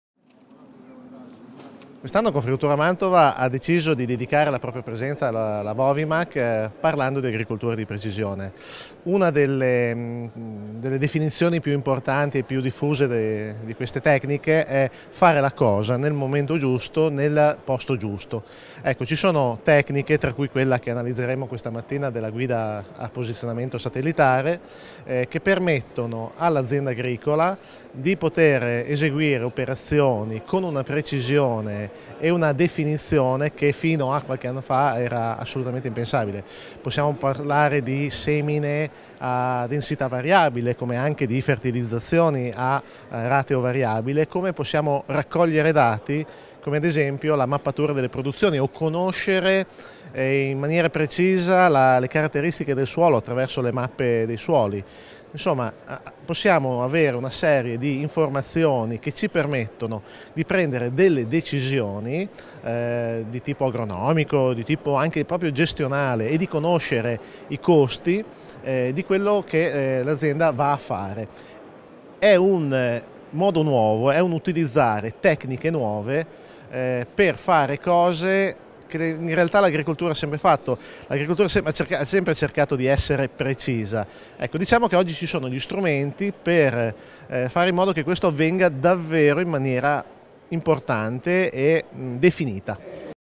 Mantova Tv - Confagricoltura Mantova a Bovimac 2019
Con la clip di Mantova.TV riviviamo i nostri appuntamenti a Bovimac 2019, con Andrea Zampolli protagonista di un focus dedicato all'agricoltura di precisione